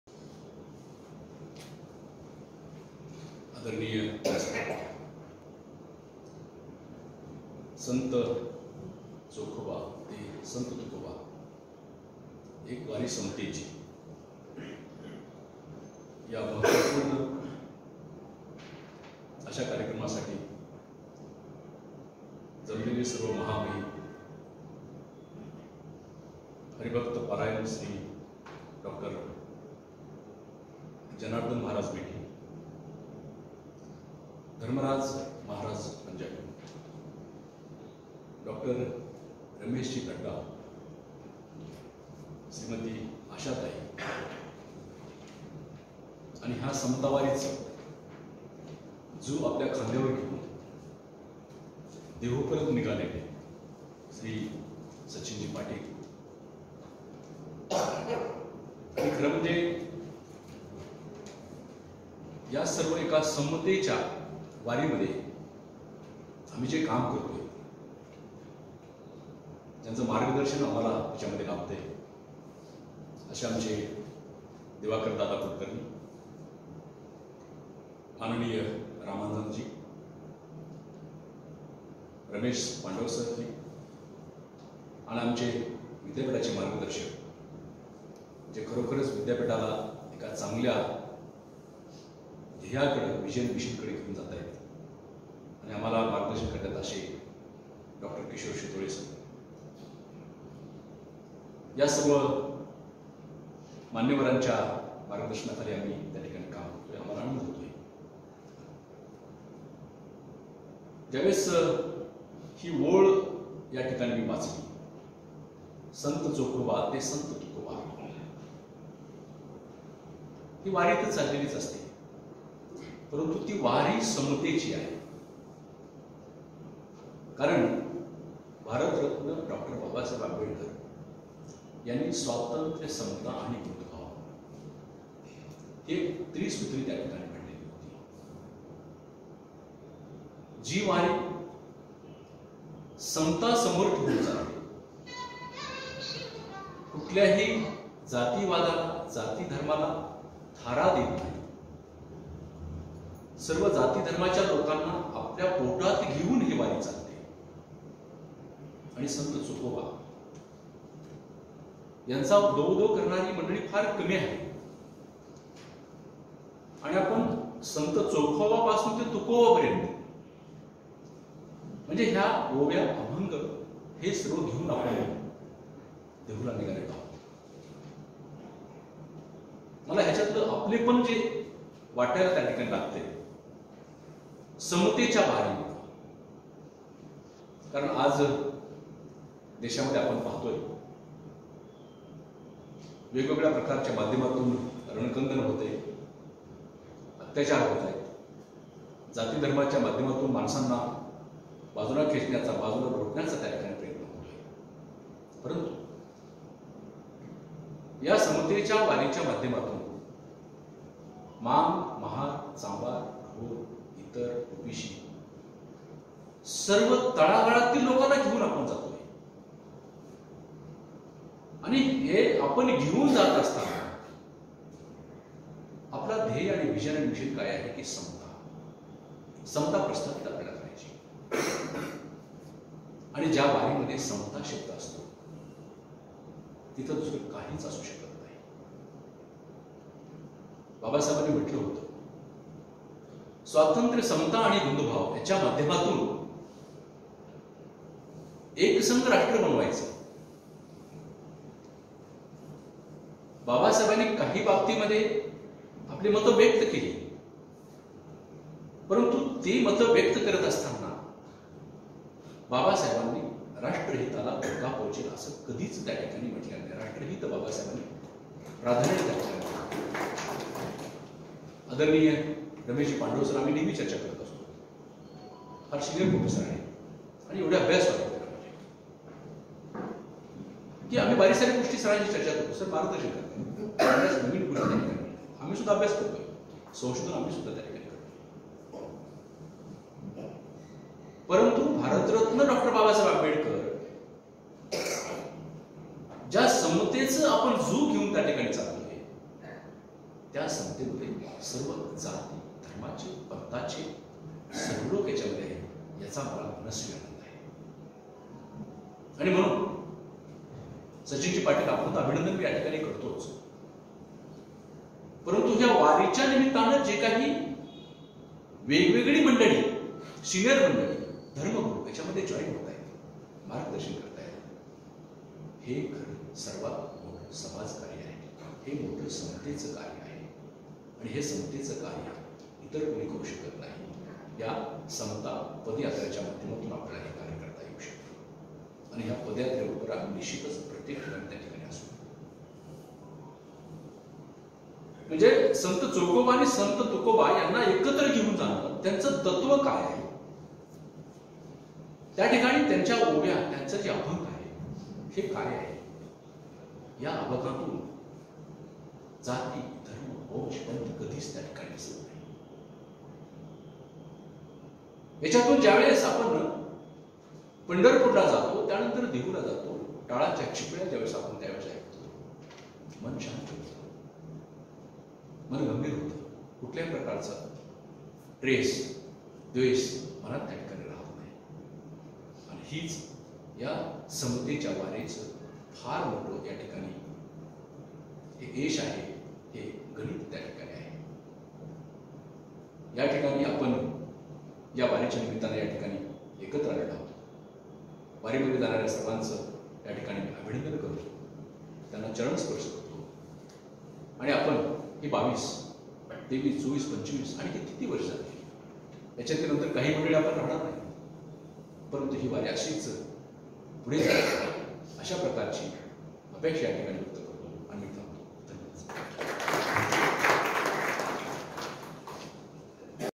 प्रा डॉ.वाल्मिक सरवदे Dr.Walmik Sarvade
"संत चोखोबा ते संत तुकोबा" एक वारी समतेची...
*संभाजीनगर शहरात समता सभेचे आयोजन*

*प्रमुख उपस्थिती -*

◆ श्री.आनंद चैतन्यजी महाराज, कन्नड
◆ प्रा.डॉ.वाल्मिक सरवदे (अधिष्ठाता, वाणिज्य व व्यवस्थापन शाखा, डॉ.बाबासाहेब आंबेडकर मराठवाडा विद्यापीठ, औ.बाद)
◆ डॉ.रमेश लड्डा (इस्कॉन केंद्र प्रमुख, संभाजीनगर)
◆ ह.भ.प.श्री डाॅ.जनार्धन महाराज मेटे (राष्ट्रीय धर्माचार्य, प्रांत संत संपर्क प्रमुख वि.हिं.प.)
◆ धर्मराज महाराज पंजाबी (सचिव ग्लोबल महानुभाव संघ)
◆ श्री.सचिन पाटील (निमंत्रक,समता वारी)

*दि. - ०६ जानेवारी २०२१ (गुरुवार)*
*वेळ - सायं ६:३० वाजता*
*स्थळ - इस्कॉन सभागृह,* इस्कॉन सिटी सेंटर,जिल्हा सत्र न्यायालय समोर, जे.जे. प्लस हॉस्पिटल जवळ, संभाजीनगर (औरंगाबाद)

समता वारीचे हे चौथे वर्ष असून, १ जानेवारी २०२२ रोजी मंगळवेढा येथील संत चोखामेळा समाधी मंदिर येथून सुरवात होऊन १२ जानेवारी २०२२ रोजी ही देहू येथे समारोप होणार आहे. ही वारी महाराष्ट्रातील ७  जिल्ह्यातुन जाणार आहे. भारतीय संविधानाला अपेक्षित समता, बंधुभाव, सामाजिक न्याय व देशाची एकता आणि एकात्मता प्रवर्धित करण्याच्या उद्देशाने संतांनी सांगितलेला समतेचा, बंधूभावाचा विचार अधिकाधिक लोकांपर्यंत पोहोचवण्यासाठी समता वारीच्या माध्यमातून प्रबोधन करण्यात येत आहे.

सूचना - कोविड १९ चे नियम पाळून सदर कार्यक्रम होणार आहे.

निमंत्रक -
- श्री.सुभाषजी शेळके (शहर प्रमुख, सामाजिक सद्भाव, संभाजीनगर)
- श्री.मोहन गोजमगुंडे (शहर प्रमुख,सामाजिक समरसता गतीविधी)